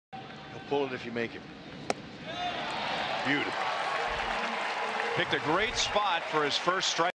CWS@KC: Vargas records first strikeout of the night
Jason Vargas gets Jose Abreu swinging to finish off his third inning of work, picking up the first strikeout of his outing